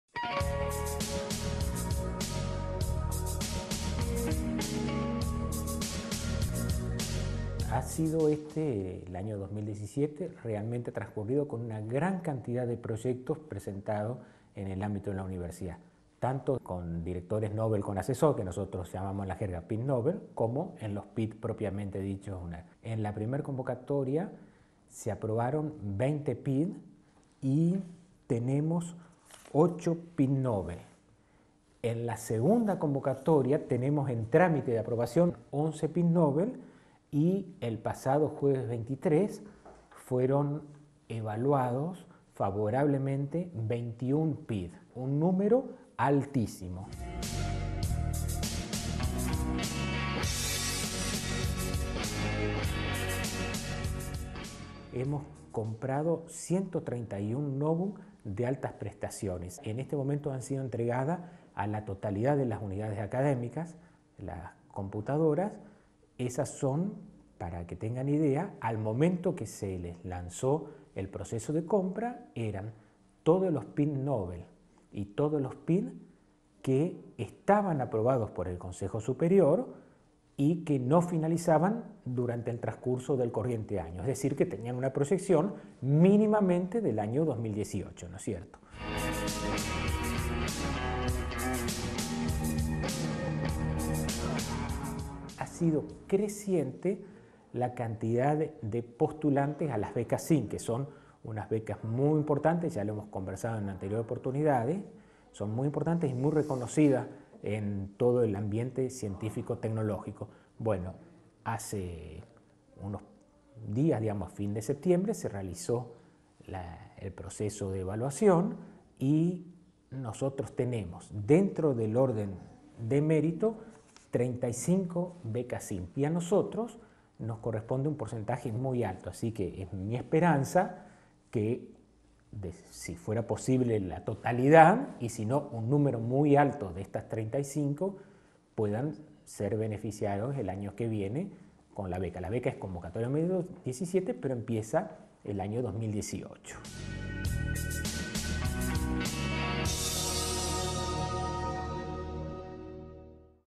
Jornada de Evaluación de proyectos de Investigación
Entrevistamos al Secretario de Ciencia y Técnica de UNER, quien nos contó las últimas novedades relacionadas al acontecer de la Secretaría. Entre ellas, la segunda evaluación de proyectos de investigación, la respuesta a la convocatoria de Becas CIN por parte de nuestros estudiantes y la adquisición de notebooks de altas prestaciones para los investigadores de la universidad.